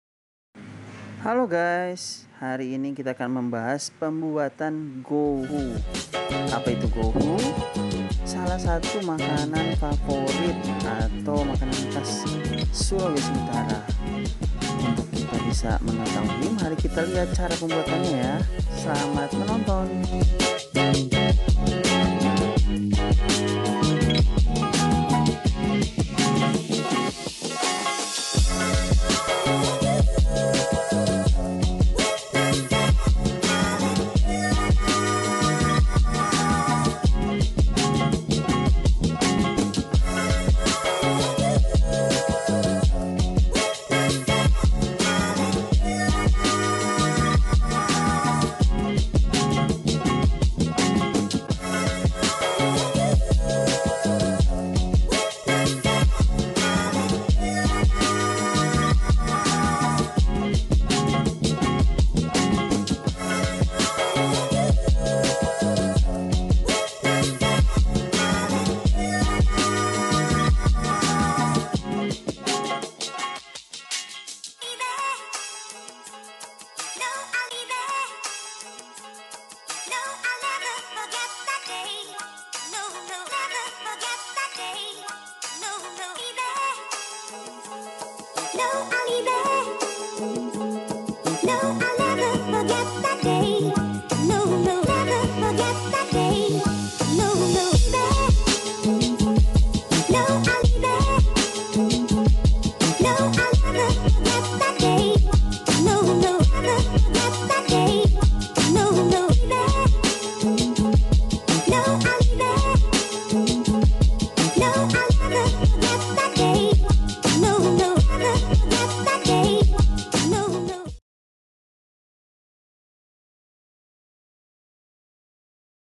Cara membuat GOHU. Makanan Khas Manado
Halo guys... kembali lagi dalam chanel youtube saya, hari ini kita membahas tentang cara pembuatan Gohu atau kalau orang jakarta sebutnya seperti asinan. Gohu ini adalah salah satu makanan khas Sulawesi Utara. Jadi kalau teman teman semua main ke Manado kalian bakal mendapat kan banyak resto atau tempat makan yang menyediakan Gohu. Tapi bagi teman teman yang tinggal di DKI jakarta jangan kwatir sekarang sudah tersedia di Jakarta. Segera kujungi IG Make'ma.